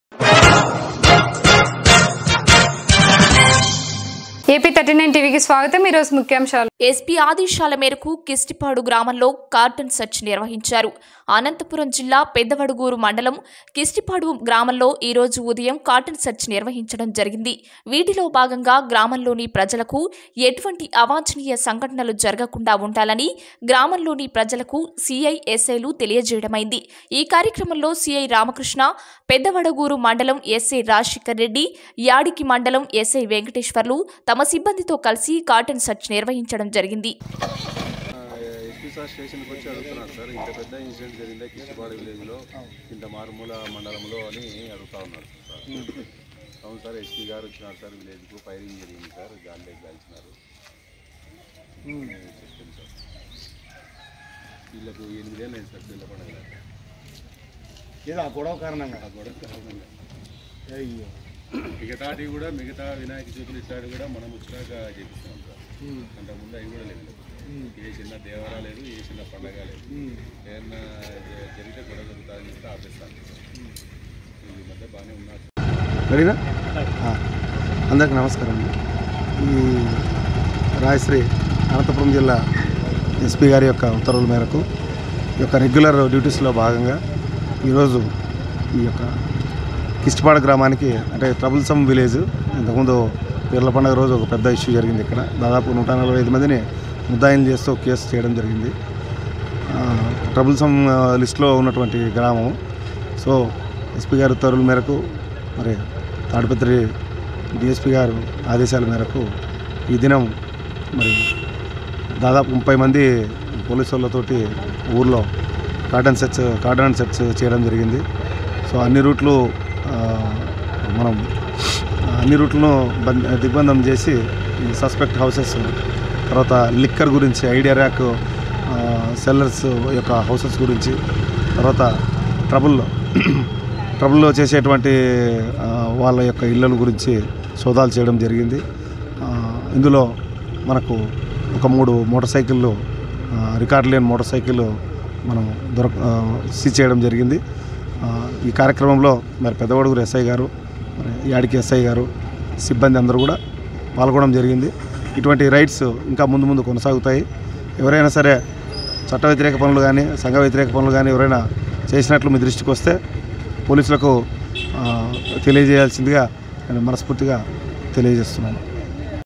ఎస్పీ ఆదేశాల మేరకు క్రిష్టిపాడు గ్రామంలో కార్టూన్ సర్చ్ నిర్వహించారు
అనంతపురం జిల్లా పెద్దవడుగూరు మండలం  క్రిష్టిపాడు గ్రామంలో  ఈరోజు ఉదయం కార్డన్ సర్చ్ నిర్వహించడం జరిగింది  వీటిలో భాగంగా  గ్రామంలోని ప్రజలకు ఎటువంటి అవాంఛనీయ సంఘటనలు జరగకుండా ఉండాలని  గ్రామంలోని ప్రజలకు  CI,SI,లు తెలియజేయడమైనది  ఈ కార్యక్రమంలో సీఐ  రామకృష్ణ పెద్దవడుగూరు మండలం ఎస్సై రాజశేఖర్ రెడ్డి  యాడికి మండలం ఎస్సై వెంకటేశ్వర్లు  మరియు తమ సిబ్బందితో కలిసి కార్డన్ సర్చ్ నిర్వహించడం జరిగింది